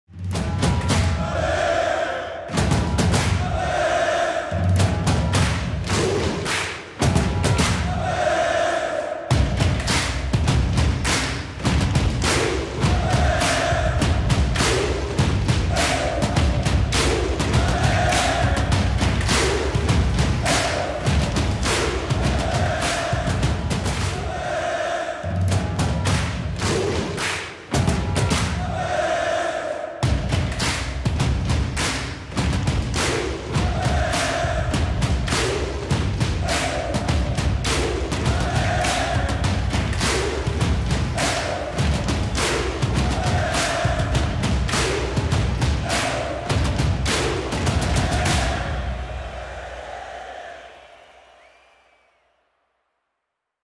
¡11 vs Junior! Con los BSCKids
Alineación vs Junior por ConmebolLibertadores, con los Socios BSC Juniors ¡Los BSCKids!